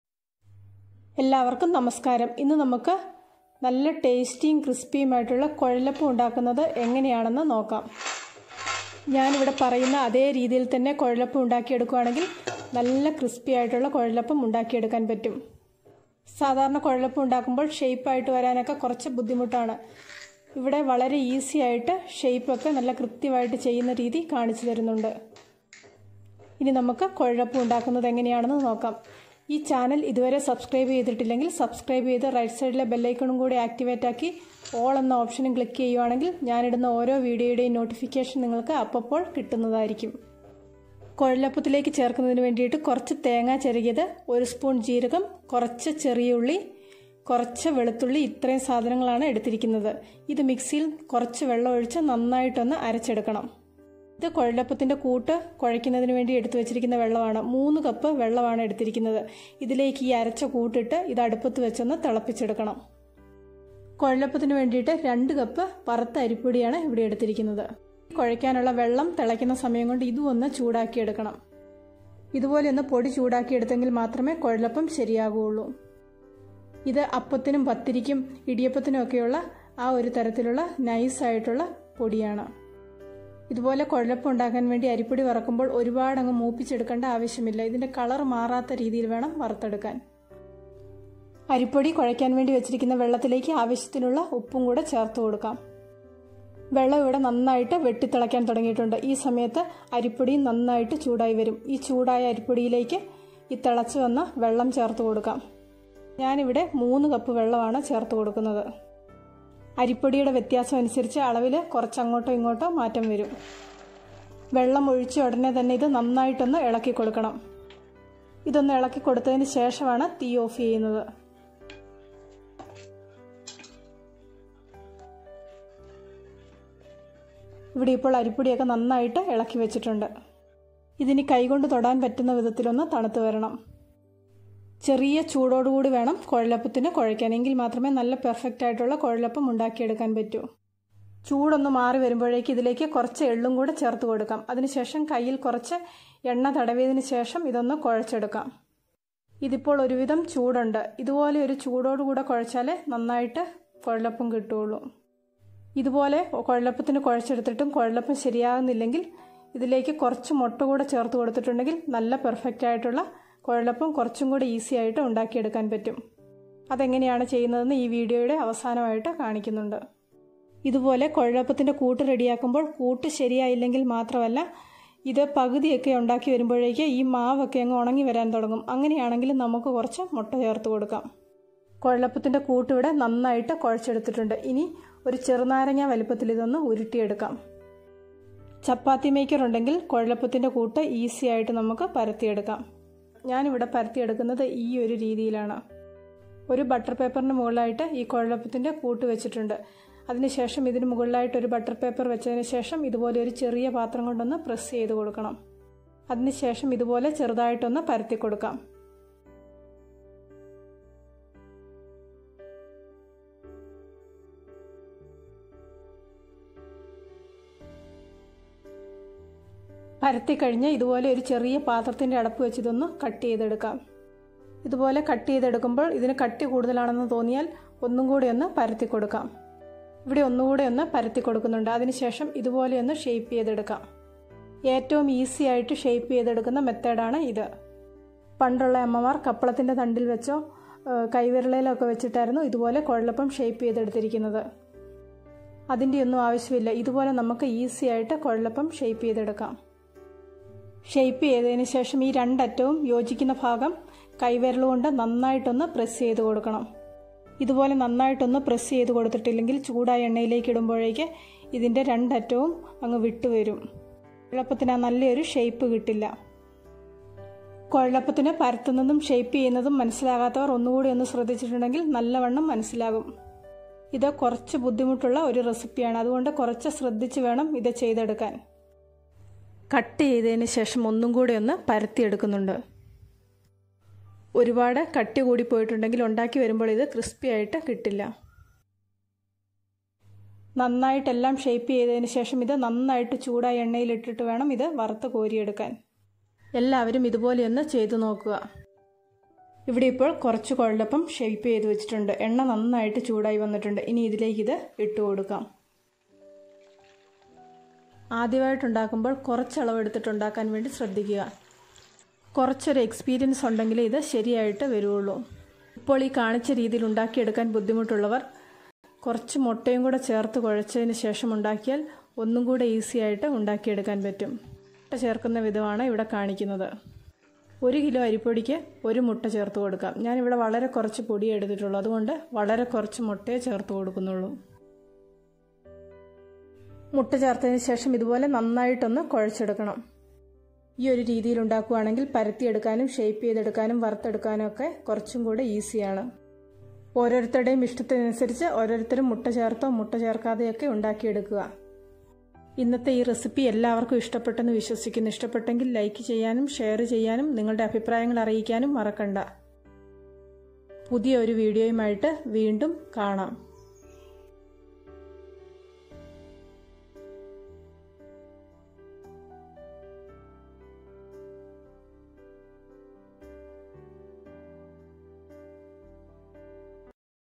കുഴലപ്പം എളുപ്പത്തിൽ ചെയ്തെടുക്കാം/ Kuzhalappam /Kuzhalappam Recipe in Malayalam/ Kerala Recipes
kerala's traditional snack recipe

ഏറ്റവും എളുപ്പത്തിൽ നല്ല പെർഫെക്ട് കുഴലപ്പം ഉണ്ടാക്കിയെടുക്കാം .

ആവശ്യമുള്ള സാധനങ്ങൾ

വറുത്ത അരിപ്പൊടി 2 കപ്പ്‌
തേങ്ങാ ചിരകിയത്  കുറച്ചു
ജീരകം  1 സ്പൂൺ
ചെറിയുള്ളി 5
വെളുത്തുള്ളി 3 അല്ലി
എള്ള്
വെള്ളം ഏകദേശ 3 കപ്പ്‌
ഉപ്പ്‌
എണ്ണ

പാചകരീതി

എള്ള് ഒഴിച്ചുള്ള ചേരുവകൾ വെള്ളത്തിൽ അരച്ച് ചേർത്ത്  തിളപ്പിക്കണം .ഈ സമയം വറുത്ത അരിപ്പൊടി ചെറു തീയിൽ ചൂടാക്കി എടുക്കണം .വെള്ളം തിളക്കുമ്പോൾ പൊടിയിലേക്ക് വെള്ളം ഒഴിച്ച് ഇളക്കി വക്കണം .വലിയ ചൂട് മാറുമ്പോൾ എള്ളും ചേർത്ത് കുഴച്ചു നാരങ്ങ വലുപ്പമുള്ള ഉരുളകളാക്കി എടുക്കണം .ശേഷം പരത്തി ഷേപ്പ് ചെയ്തു ചൂടായ എണ്ണയിൽ വറുത്തെടുക്കാം .പരത്തുമ്പോഴും ഷേപ്പ് ചെയ്യുമ്പോഴും ഒരുപാട് കാര്യങ്ങൾ ശ്രദ്ധിക്കണം .ഈ ഒരു കൂട്ടിൽ കുഴലപ്പം ശരിയാകുന്നില്ലെങ്കിൽ കുറച്ച് മുട്ടയും കൂടി ചേർത്ത് കുഴച്ചാൽ പെർഫെക്ട് കുഴലപ്പം കിട്ടും .കൂടുതൽ വിഡിയോയിൽ